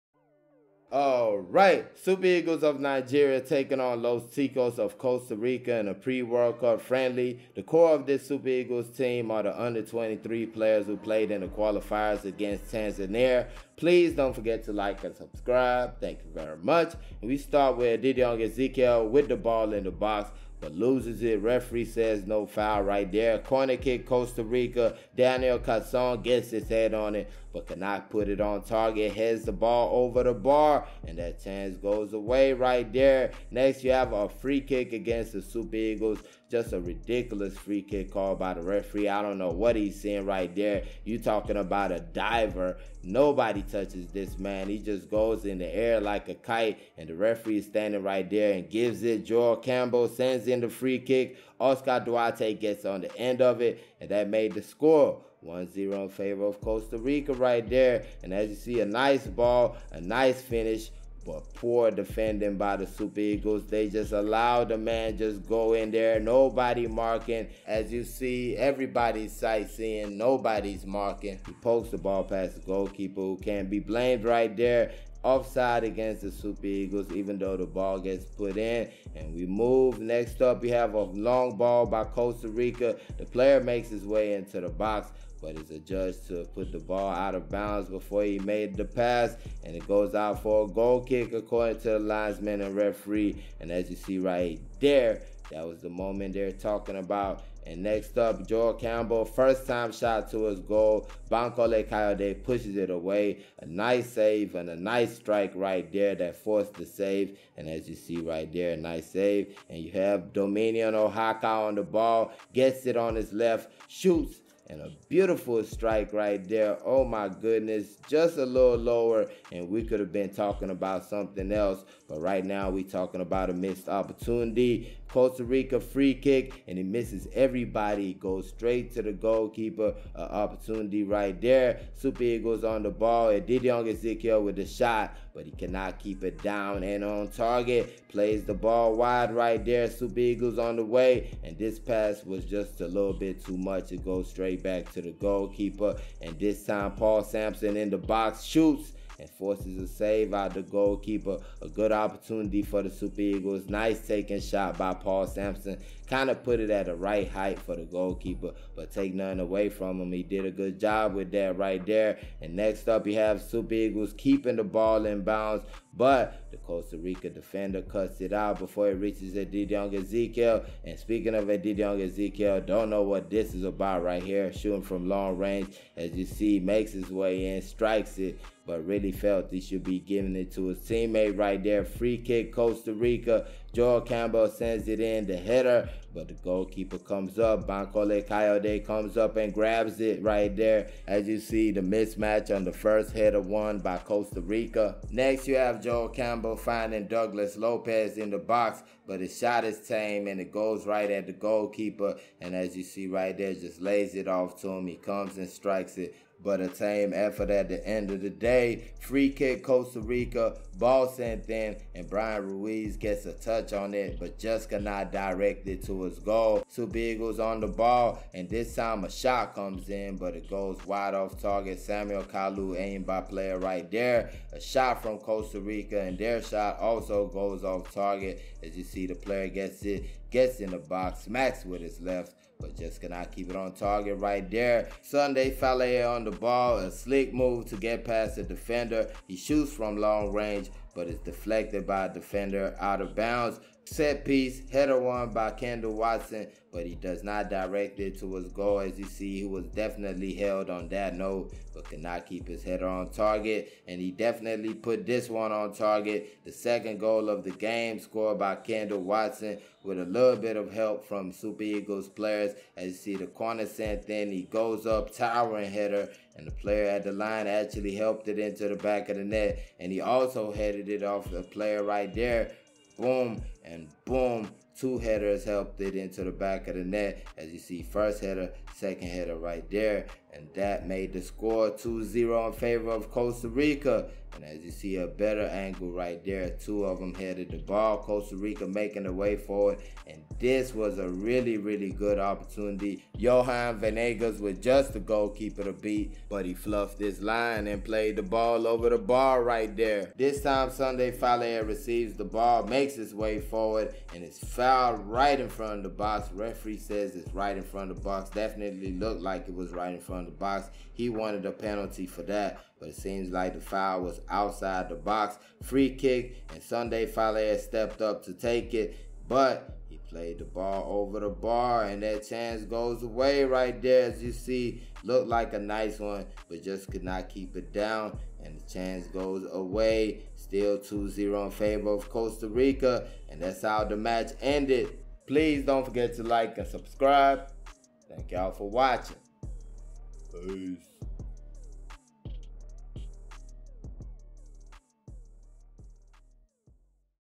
Nigeria vs Costa Rica [0-2] Friendly Match 2022 | Full highlights
Nigeria Super Eagles B team were defeated by Los Ticos of Costa Rica in an international friendly match. The Super Eagles squad was made out of players from the local league (NPFL) while Costa Rica deployed their main squad who are preparing for the 2022 FIFA World Cup in Qatar.